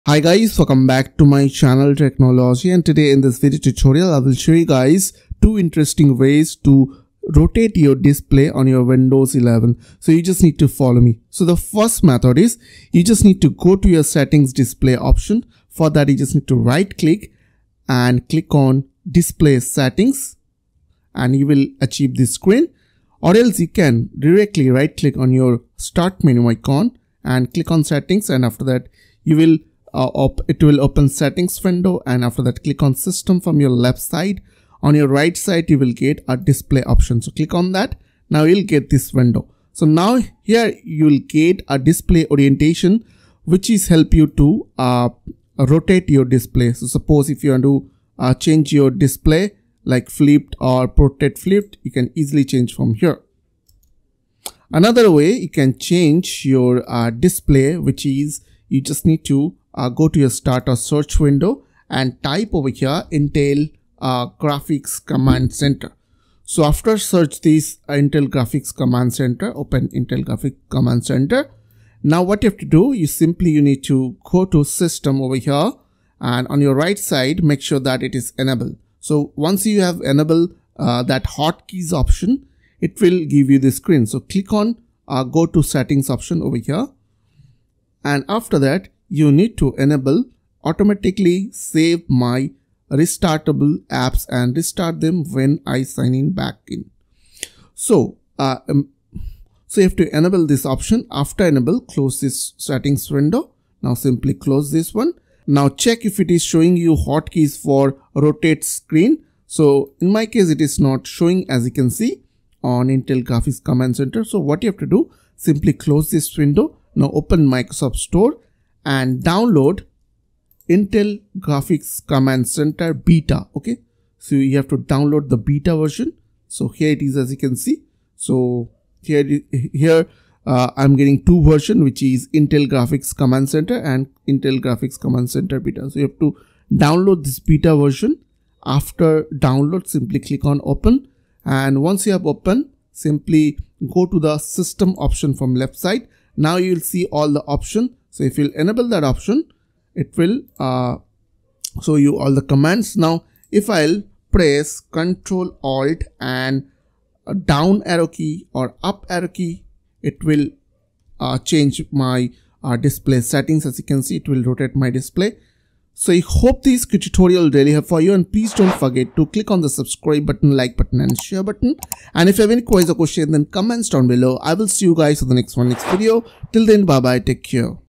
2 Simple Ways to Rotate Your Windows 11 Screen
If you want to quickly and easily rotate your screen on Windows 11, Whether it's for your reading experiences, coding or troubleshooting. This video will help you to rotate screen 2 easy methods using just a few clicks.

💻What You will Learn:

How to change the display orientations using Windows 11 Settings.
1. Enable Hotkeys for Screen rotation.
2. Rotate Screen using keyboard shortcuts.

💻Provided Solutions For:

1. Screen rotate hotkeys not showing.
2. Install Intel Graphics command centre.

Timestamps:

00:00 Intro
00:11 Change Display Orientations Using Settings
00:54 Change Display Using Hotkeys